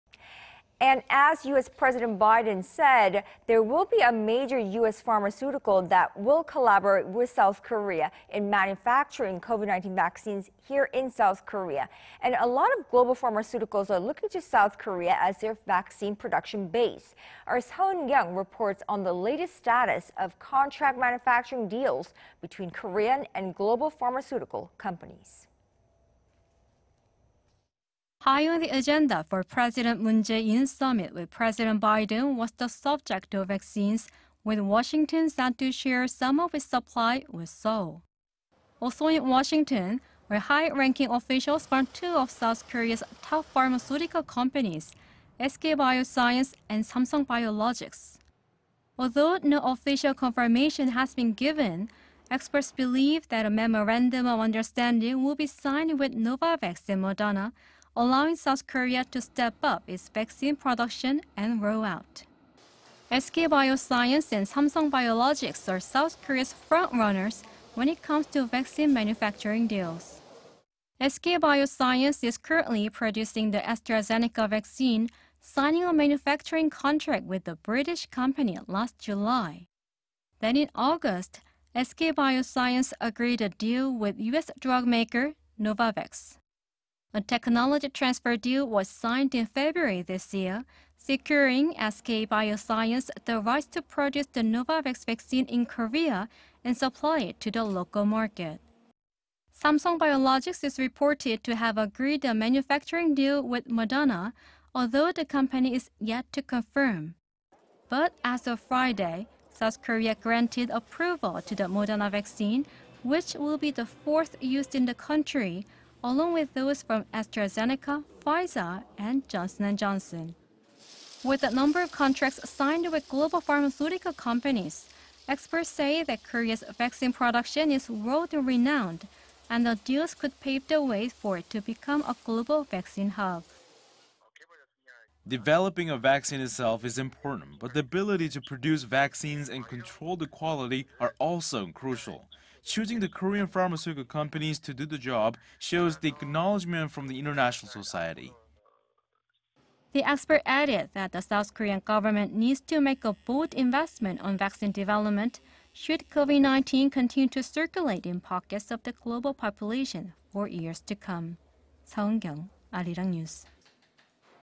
Latest updates on contract manufacturing deal on vaccines in South Korea
한국의 위탁생산현황 어디까지 왔나...위탁생산이 갖는 의미는
Global pharmaceutical companies are focusing on South Korea as a COVID-19 vaccine production base.
Seo Eunkyung reports the latest status of contract manufacturing deals between Korean and global pharmaceutical companies.

High on the agenda for President Moon Jae-in's summit with President Biden was the subject of vaccines... with Washington set to share some of its supply with Seoul.
Also in Washington are high-ranking officials from two of South Korea's top pharmaceutical companies, SK Bioscience and Samsung BioLogics.
Although no official confirmation has been given, experts believe that a memorandum of understanding will be signed with Novavax and Moderna... allowing South Korea to step up its vaccine production and rollout.

SK Bioscience and Samsung BioLogics are South Korea's front runners when it comes to vaccine manufacturing deals.
SK Bioscience is currently producing the AstraZeneca vaccine, signing a manufacturing contract with the British company last July.
Then, in August, SK Bioscience agreed a deal with U.S. drugmaker Novavax.
A "technology transfer" deal was signed in February this year   securing SK Bioscience the rights to produce the Novavax vaccine in Korea and supply it to the local market.
Samsung BioLogics is reported to have agreed a manufacturing deal with Moderna... although the company is yet to confirm.
But, as of Friday, South Korea granted approval to the Moderna vaccine, which will be the fourth used in the country... along with those from AstraZeneca, Pfizer and Johnson and Johnson.

With a number of contracts signed with global pharmaceutical companies, experts say that Korea's vaccine production is world-renowned, and the deals could pave the way for it to become a global vaccine hub.

"Developing a vaccine itself is important, but the ability to produce vaccines and control the quality are also crucial. Choosing Korean pharmaceutical companies to do the job shows the acknowledgement from the international society."

The expert added that the South Korean government needs make a bold investment on vaccine development, should COVID-19 continue to circulate in pockets of the global population for years to come.
SEO Eunkyung Arirang News.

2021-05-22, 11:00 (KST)